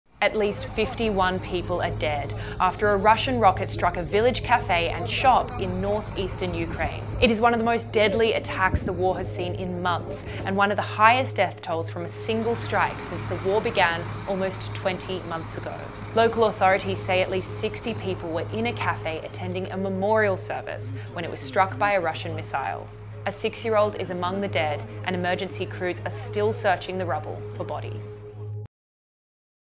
51 dead in Russian missile strike
At least 51 people are dead, after a Russian rocket struck a village cafe and shop in eastern Ukraine.